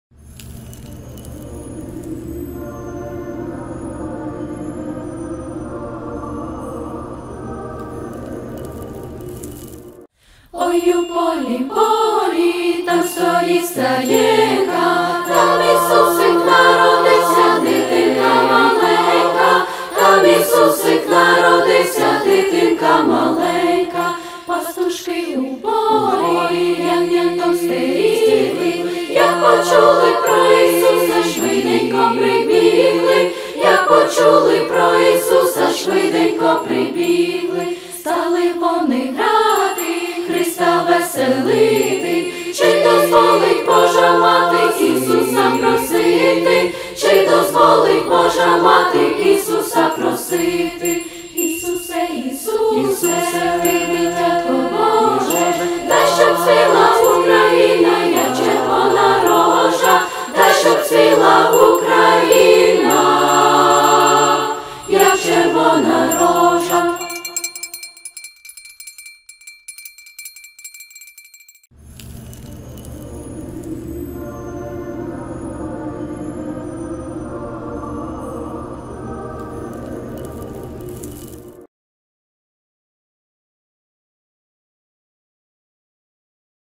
Колядки з бомбосховищ — зворушливий різдвяний проєкт
Суспільне представляє зворушливий проєкт до Різдва Христового та Нового року — «Колядки з бомбосховищ». Діти та дорослі з різних куточків України заспівали різдвяних пісень у сховищах, де вони проводять багато часу через російські обстріли. Ми в Тернополі обрали для цього проєкту хор Тернопільського ліцею №21-спеціалізованої мистецької школи імені Ігоря Ґерети. Відео зняте в укритті школи.